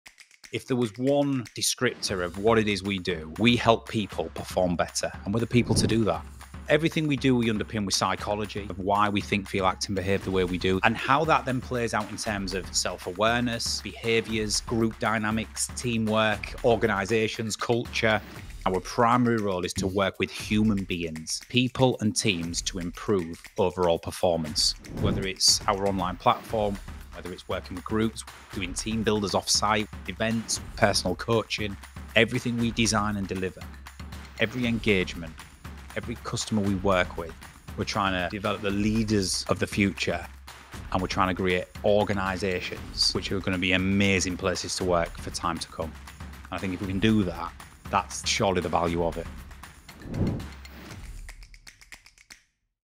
Trans2 Performance: The People Performance People
Trans2 Performance is a leading people performance company that helps people, teams, and organisations perform better. We do this by providing a range of services, including:

- Leadership development
- Team building
- Performance coaching
- Organizational culture change
- Online learning platform

We are passionate about helping people reach their full potential and creating amazing workplaces. We believe that when people are performing at their best, it has a positive impact on everyone involved, from the individual to the team to the organisation as a whole.

If you are looking for a way to improve your performance, your team's performance, or your organisation's performance, then Trans2 Performance can help.